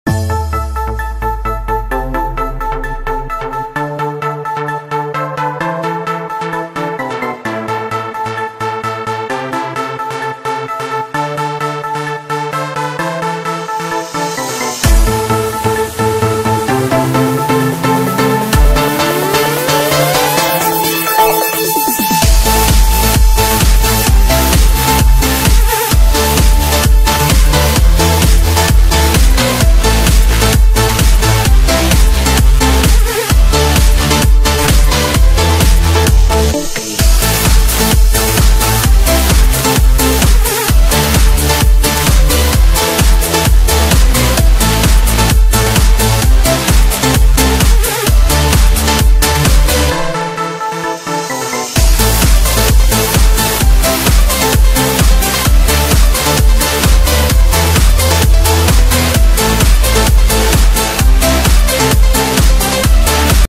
[4K 60fps] Volume Uprise by Snowbound
Level Info:

Level Name: Volume Uprise
Level ID: 10075121
Level Author: Snowbound
Level Difficulty: Harder 7★